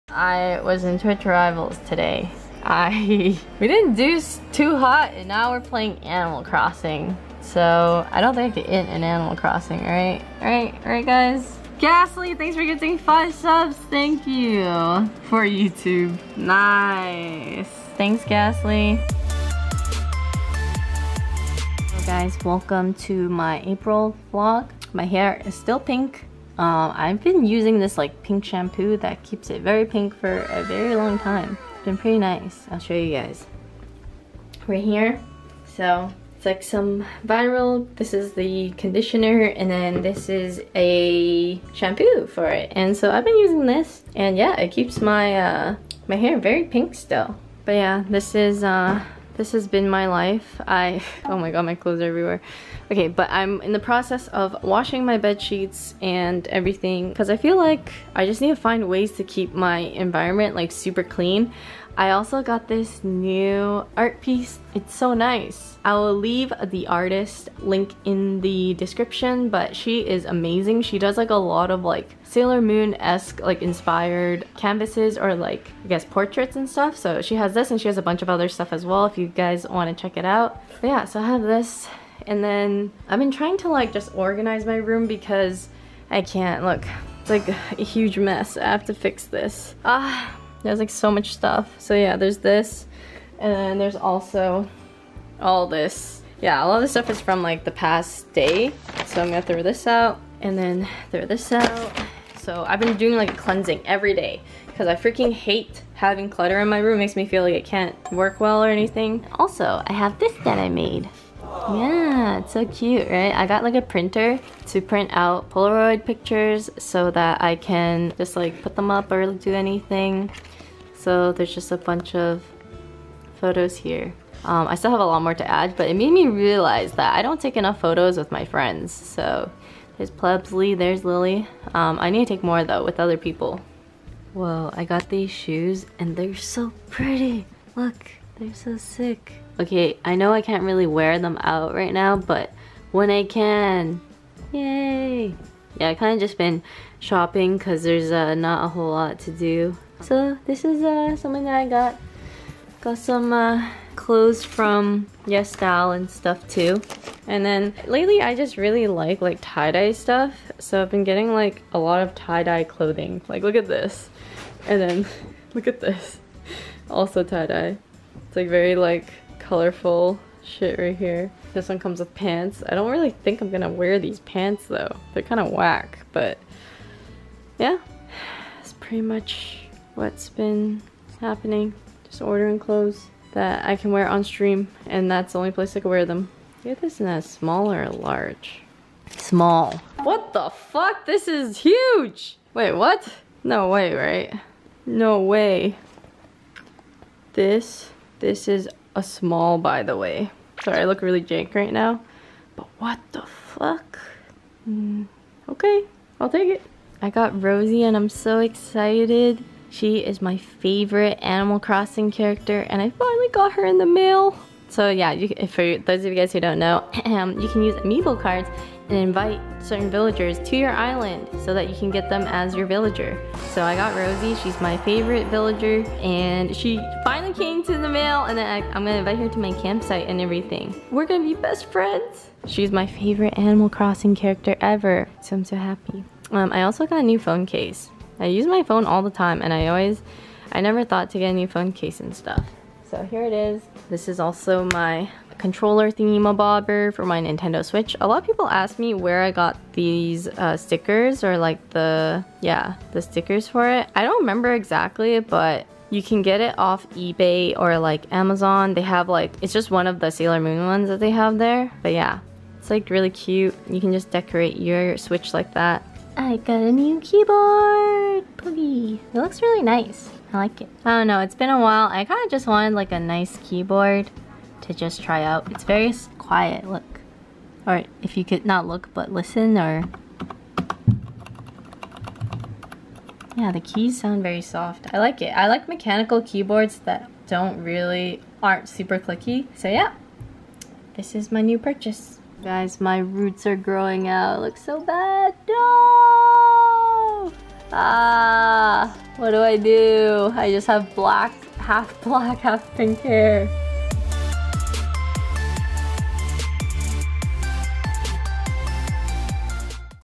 Yvonne APRIL VLOG
small april life updates! not a whole lot cause can't leave the house but hope you guys enjoy regardless :D

Click! 🔻